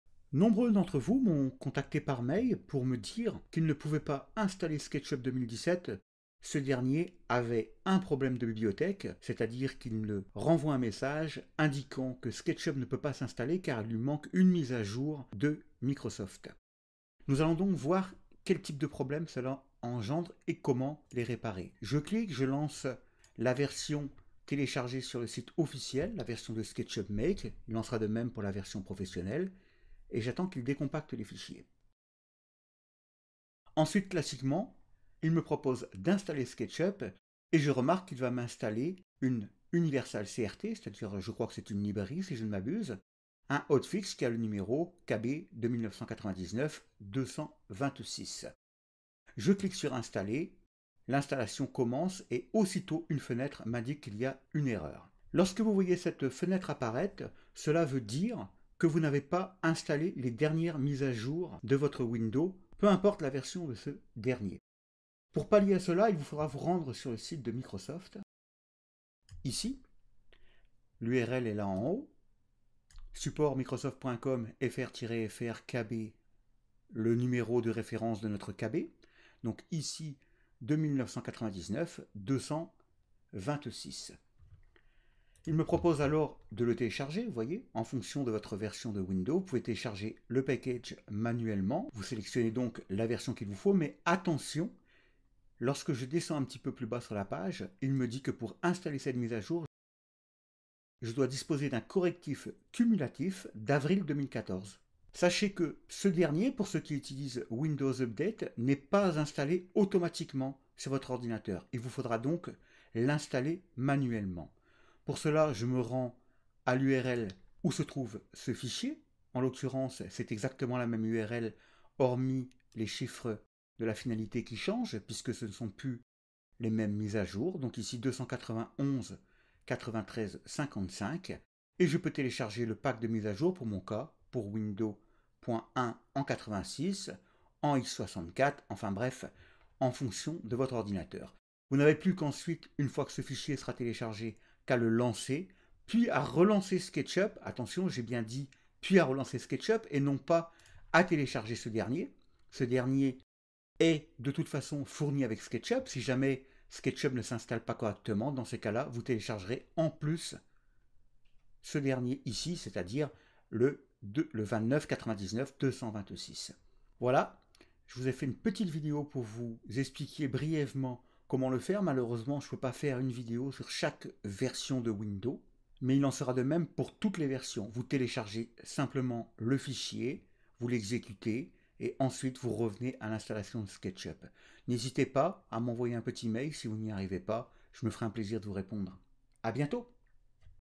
Sketchup 2017 Problème d'installation -Hotfix KB2999226-
Résoudre le problème d'installation de Sketchup 2017 relatif à l'erreur KB-2999226 en français et en vidéo :)